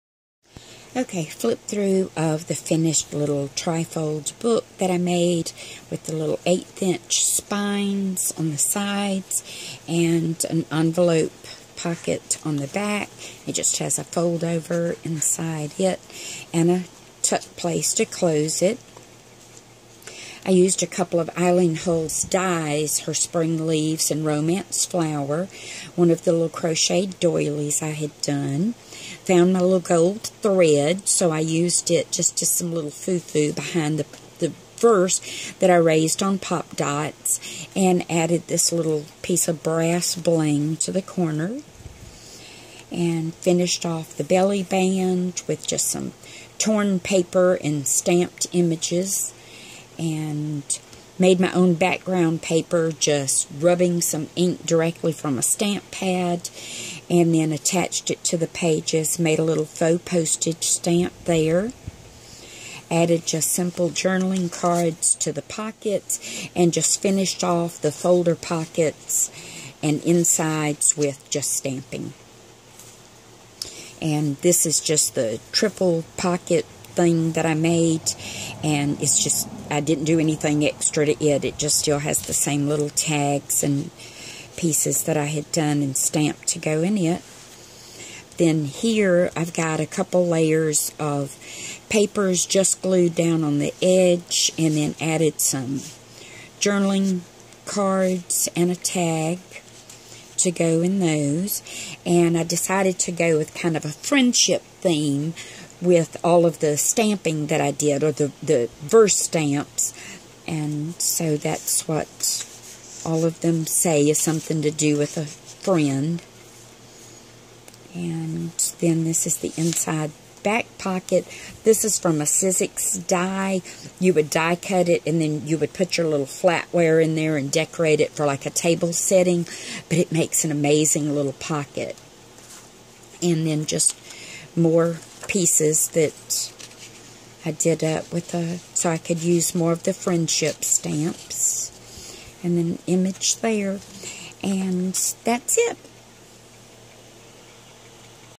Friendship Folio Journal Booklet
A little tri fold junk journal style folio I designed and made